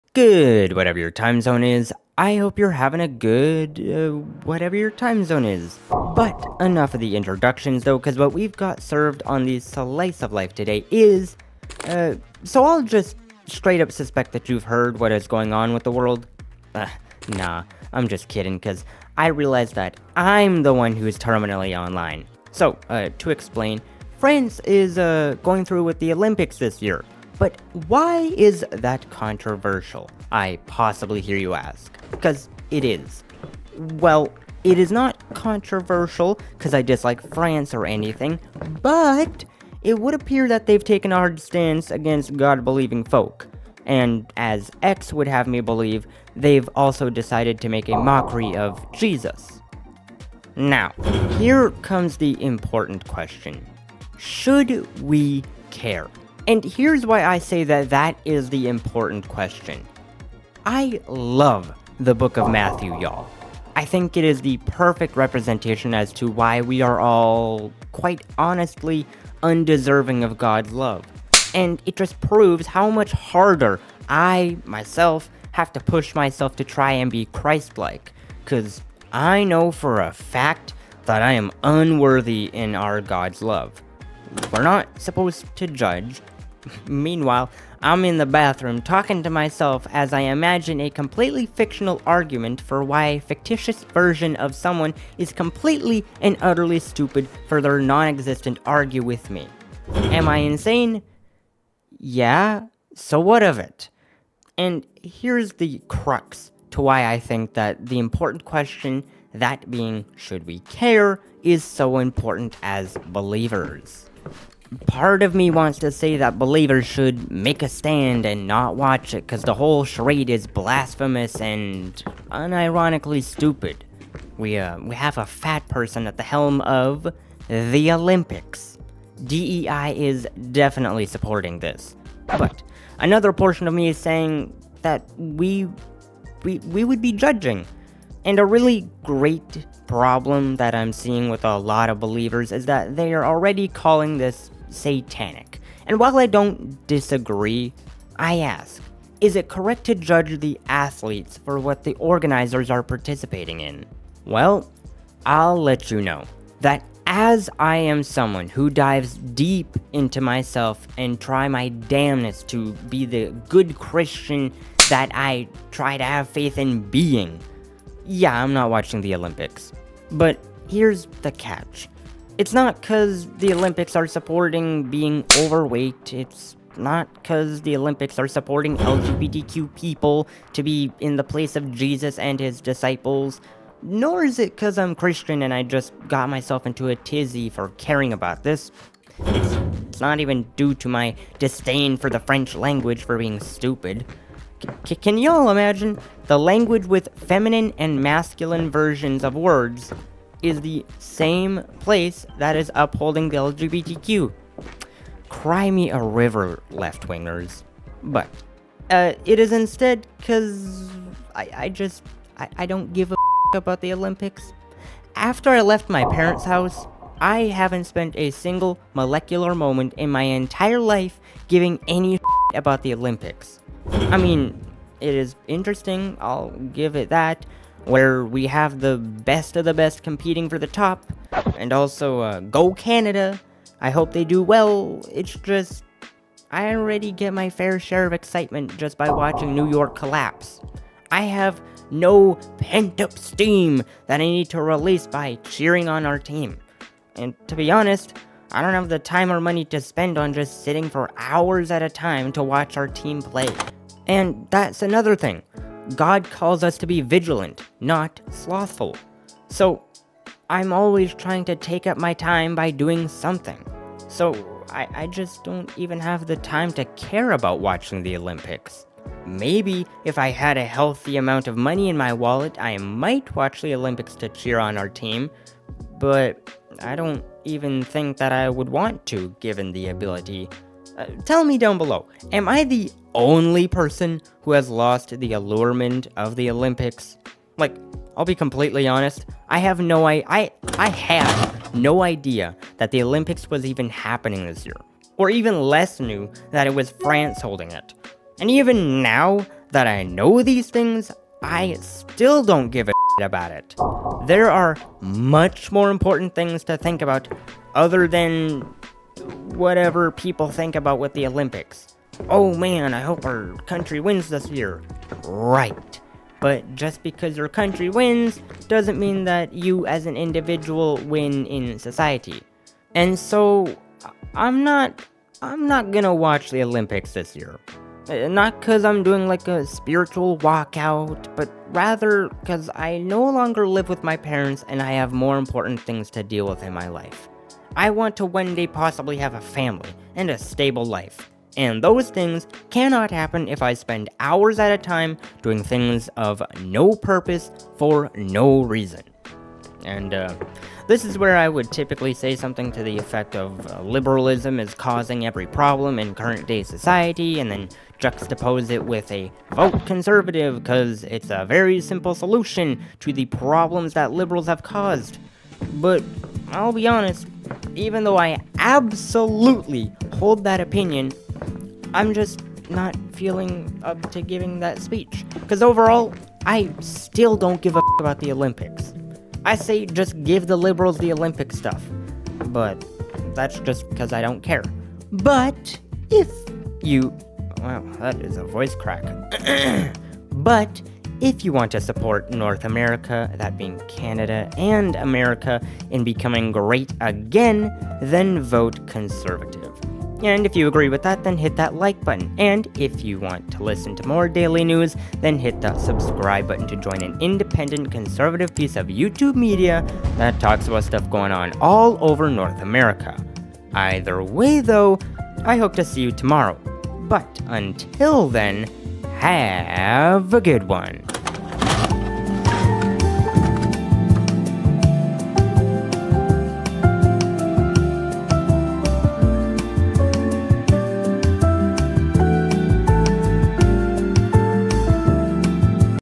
Aren't the Olympics about Sports? Now Introducing; The ETERNAL Limbo!!
In France, A ONCE in a lifetime dance is happening between the passage of Heaven and Eternal Damnation… I feel like somethin’ is coming to Earth in our lifetime folks… Christ is King!
Across The Pond Politics #2

Credit: X

Original video(s): - X

Chapters:
00:00 Hope your timezones goin' good!
00:27 Why are the Olympics Controversial?
00:50 A REALLY Important Question
03:25 My VERY Interesting Opinion
04:29 Would I Care if THIS Happened?